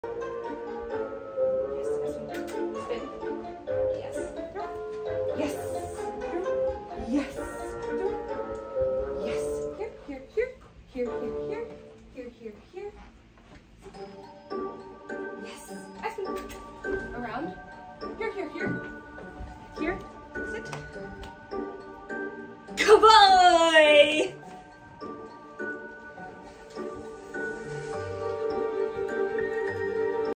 Aspen PFS Pumpkin Ball Performer Title Submission
No treats in hand
3 connected behaviors: spin, leg weaves, circle with handler
Around the Halloween decoration
Closing behavior: peekaboo through handlers legs